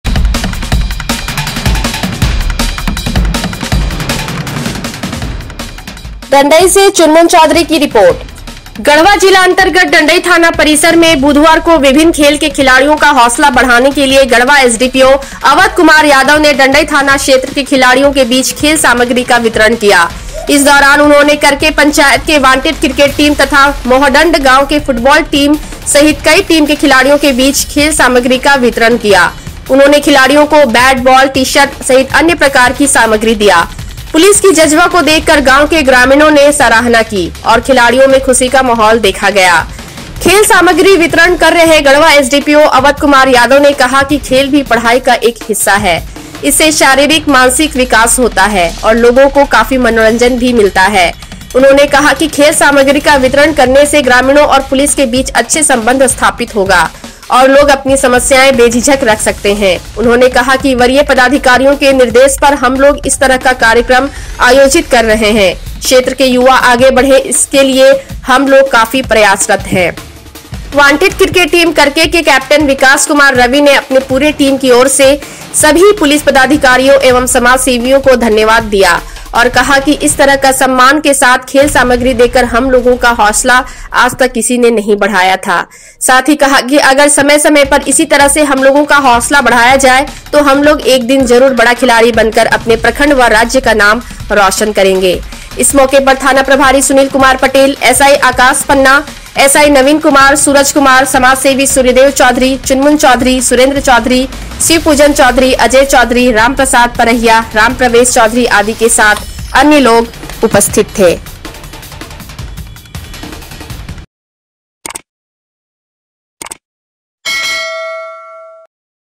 Dandai - थाना परिसर में खिलाड़ियों को खेल सामग्री देकर पुलिस ने बढ़ाया हौसला
dandai news , breaking news dandai , dandai garhwa news , dandai breaking news